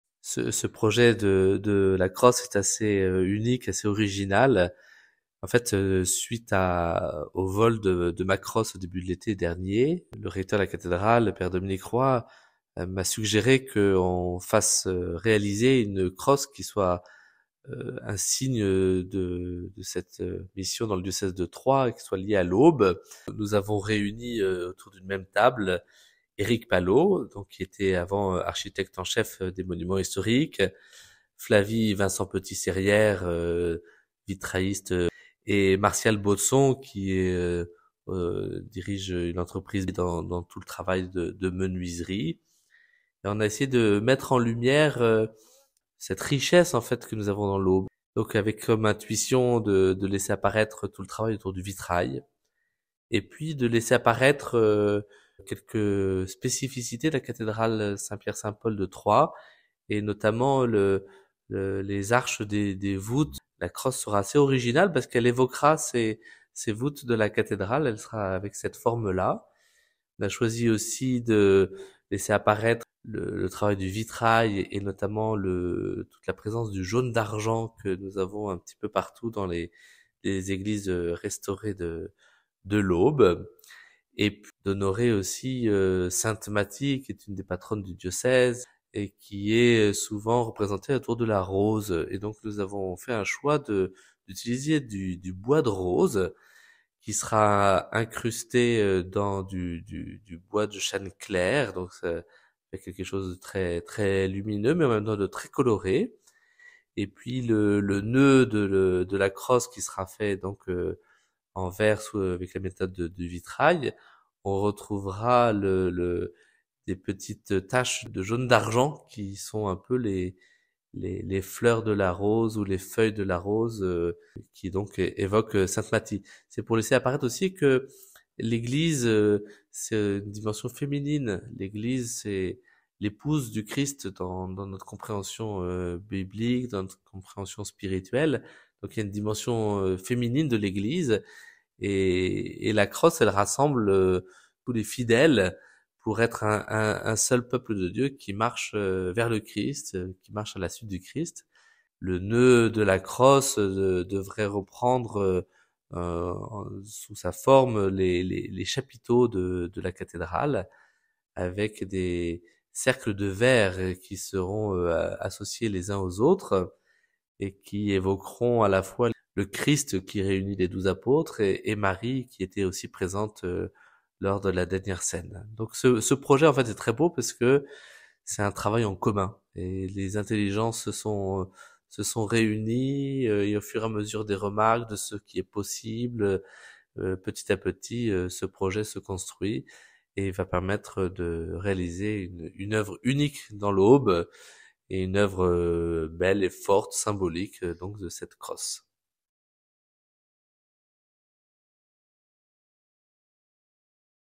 Une nouvelle crosse pour notre évêque
Suite au vol de sa crosse épiscopale, Monseigneur Alexandre Joly a réuni plusieurs artisans locaux afin d'en fabriquer une nouvelle. L'originalité du projet ? La crosse sera auboise !

Si vous souhaitez participer à la souscription de la nouvelle crosse de Monseigneur Alexandre Joly, vous pouvez déposer votre chèque à la cathédrale ou l'adresser à :
Cathédrale de Troyes
Evêché de Troyes
3 rue du Cloître Saint Etienne
10000 Troyes